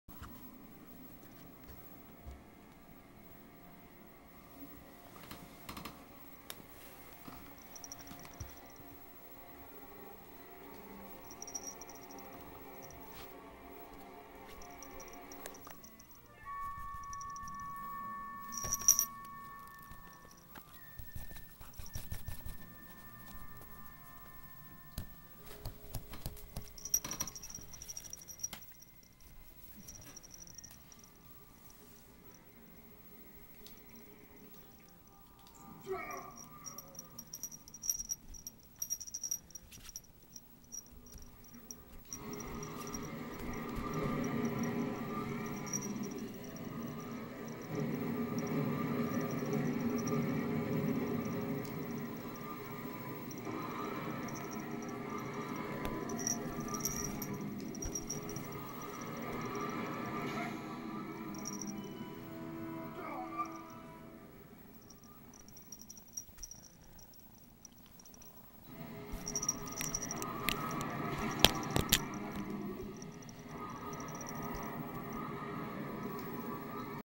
Palette being adorable!
Palette "Itty Bitty" just being her regular cute kitten self. =)